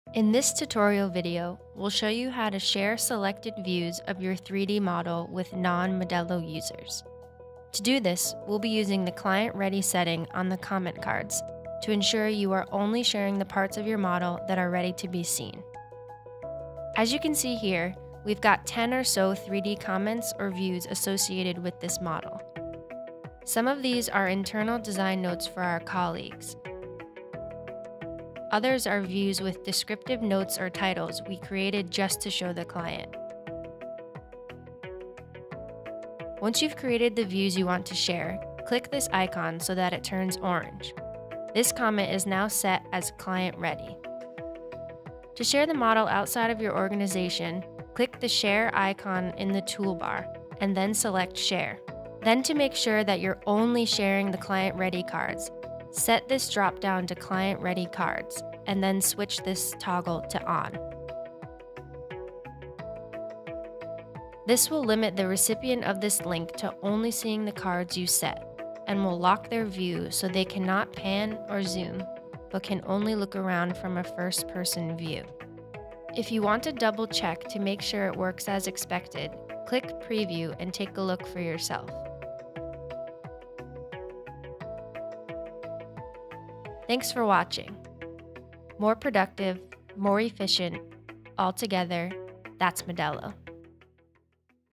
Modelo 3D tutorial/ 3D Comments/ Client Ready
In this tutorial video we’ll show you how to share selected views of your 3D model with non-Modelo users.

To do this we’ll be using “Client-Ready” setting on the comment cards to ensure you are only sharing the parts of your model that are ready to be seen.

As you can see here we’ve got 10 or so 3D comments or views associated with this model.

Some of these are internal design notes for our colleagues, others are views with descriptive notes or titles we created just to show the client. Once you’ve created the views you want to share, click this icon so that it turns orange. This Comment is now set as “client-ready.”

So now what? To share the model outside of your organization click the Share icon in the toolbar, and then select “Share”

Then to make sure that you’re only sharing the client-ready cards - set this dropdown to “Client Ready Cards” and then switch this toggle to “on”.

This will limit the recipient of this link to only seeing the cards you  set and will lock their view so they cannot pan or zoom but can  only look around from a first person view. If you want to double check to make sure it works as expected, click preview and take a look for yourself.